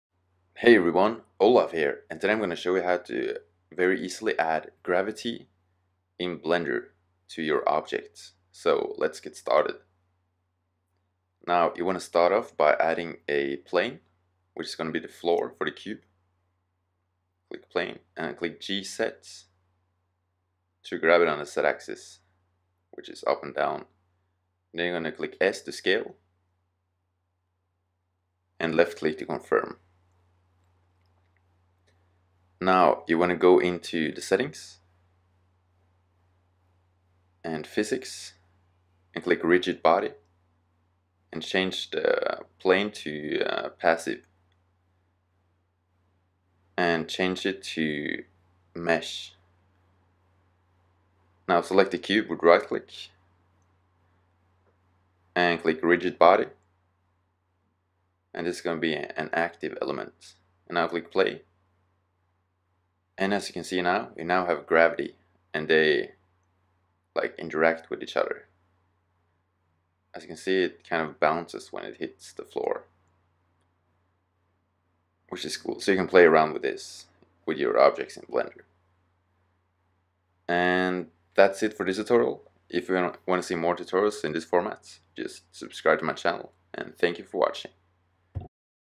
[2.79] How To Quickly Add Gravity To Objects In Blender
In this quick tutorial you will learn about gravity in Blender.